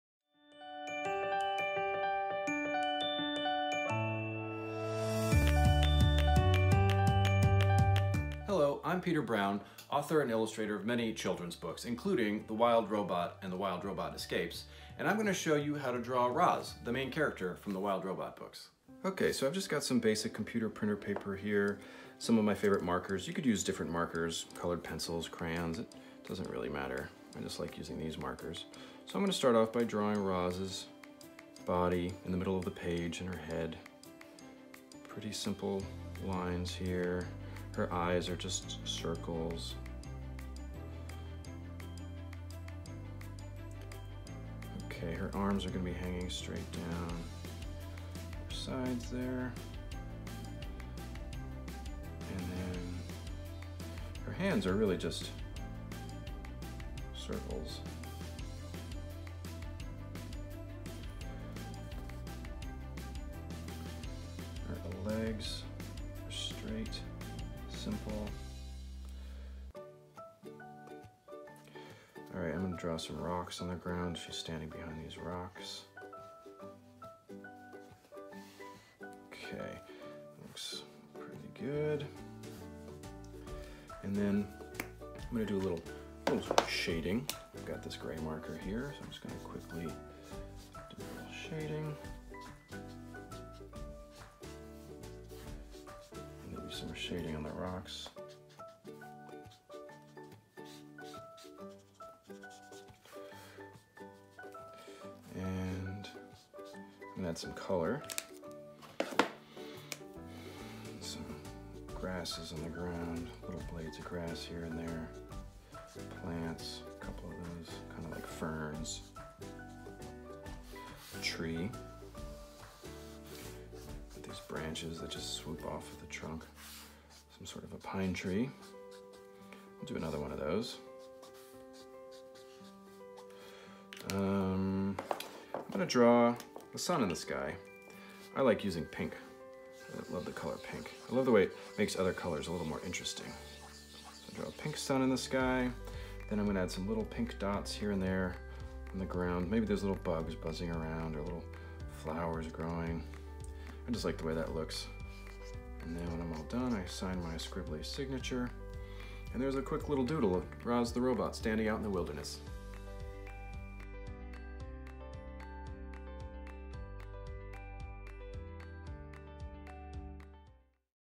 LBYR Draws! The Wild Robot
Join author/illustrator Peter Brown to learn how he draws Roz from THE WILD ROBOT!